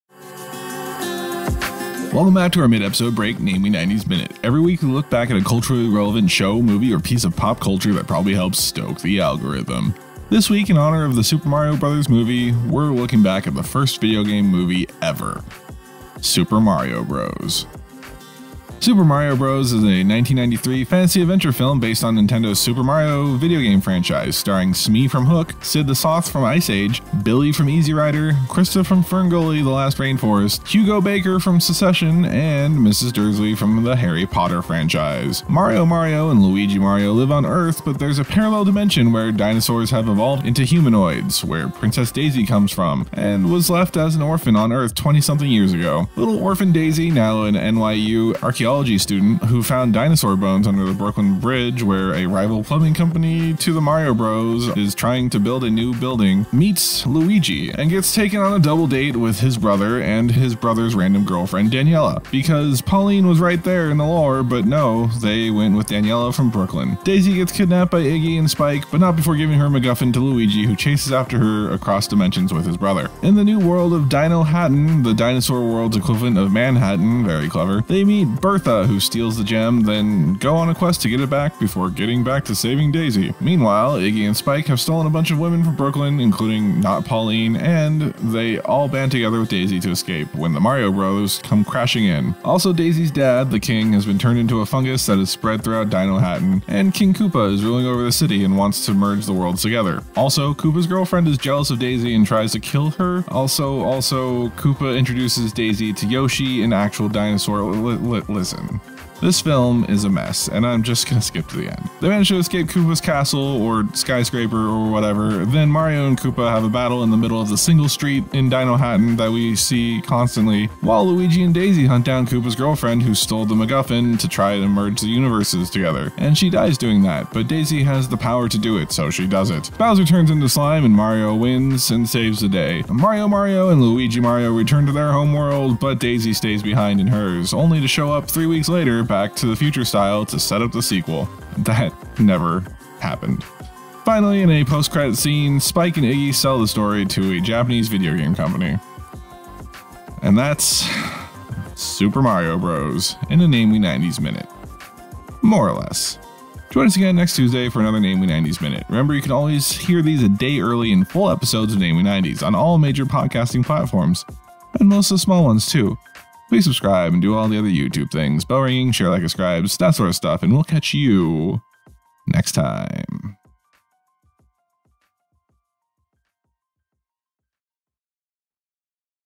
Super Mario Bros. movie (in a Namely 90s Minute)
In honor of The Super Mario Bros. Movie, we're looking back at the first video game movie... Super Mario Bros. from 1993!